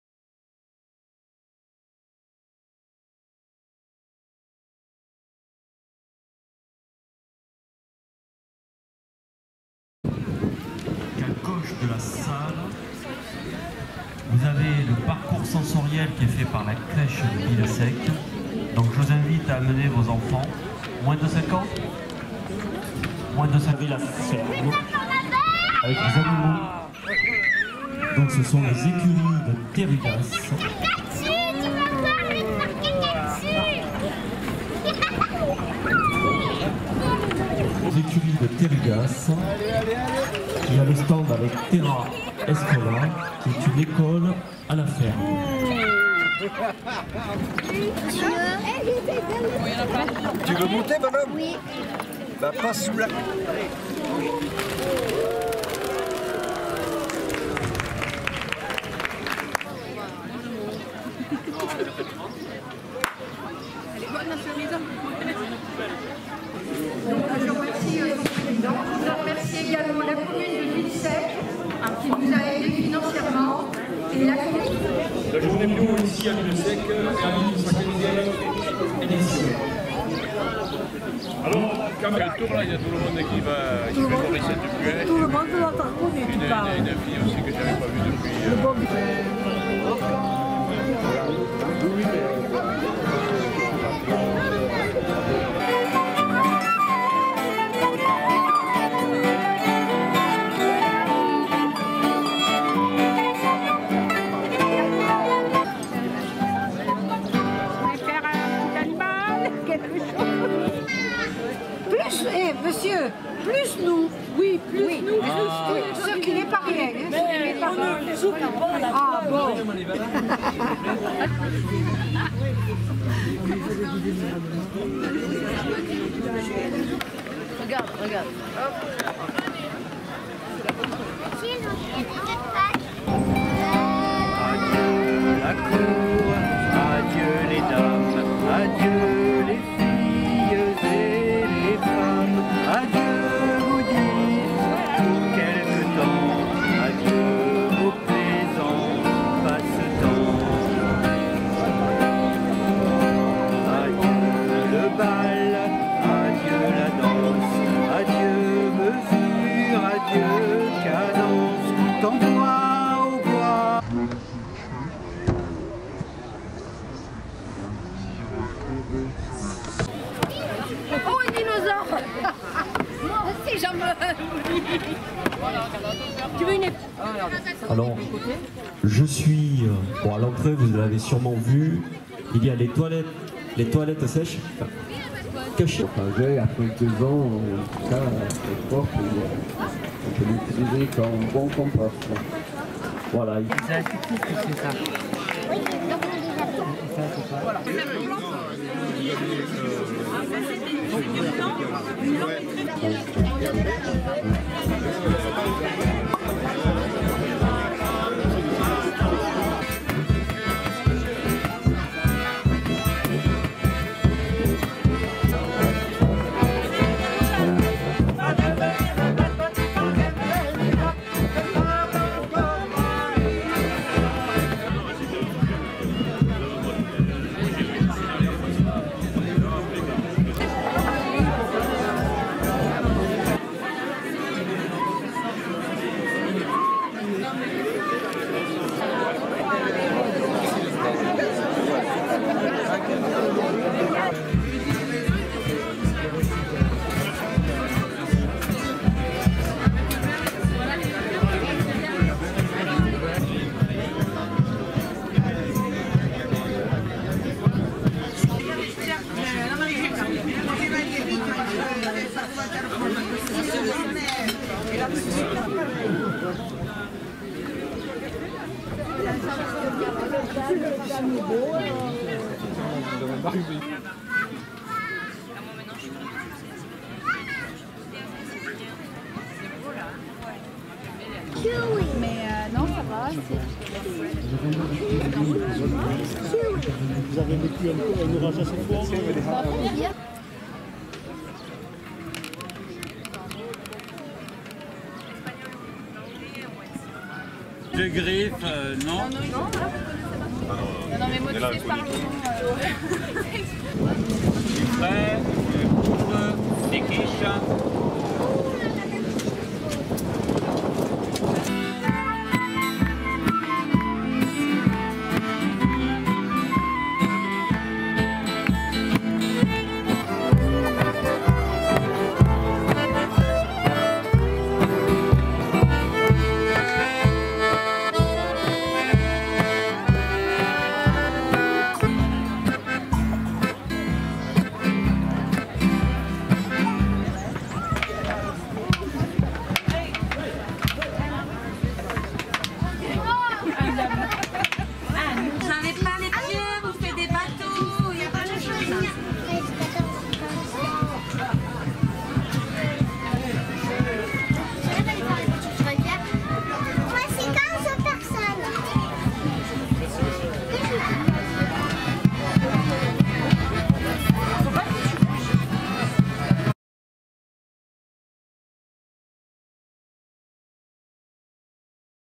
Journée Bio Villesèque des Corbières (4KULTRA HD )
La journée Bio des Corbières inscrite au Printemps Bio , réalise cette année sa 15éme édition . Cette manifestation présente une soixantaine de producteurs travaillant des matières naturelles, mais aussi des stands associatifs et des animations diverses . Premier film